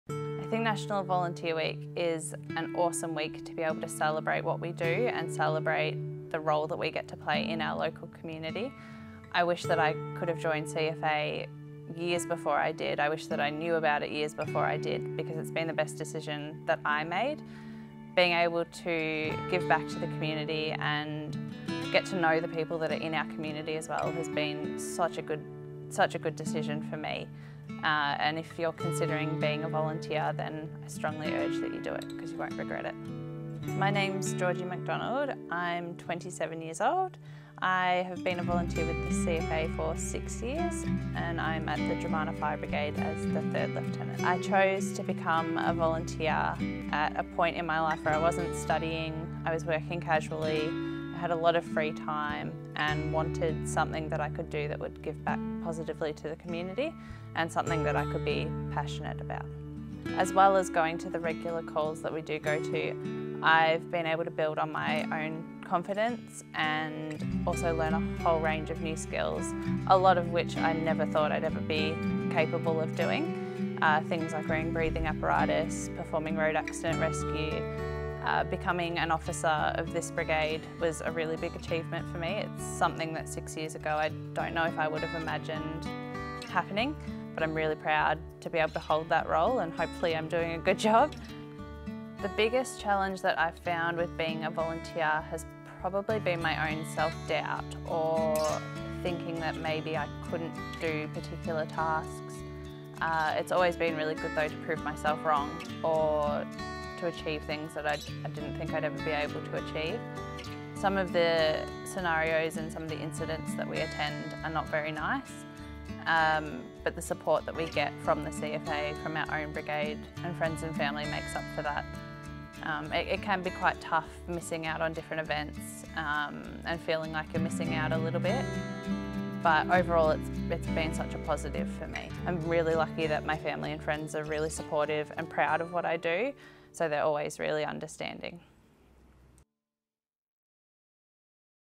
Georgina McDonald - National Volunteer Week 2019
Ahead of National Volunteer Week, we spoke to Georgie who tells us why she loves volunteering with CFA.
Georgie was also a finalist in the Seven News Young Achiever Awards VIC and like many of our volunteers reflects the spirit and values of CFA. This week we say THANK YOU to all our volunteers who dedicate their time to serving their communities.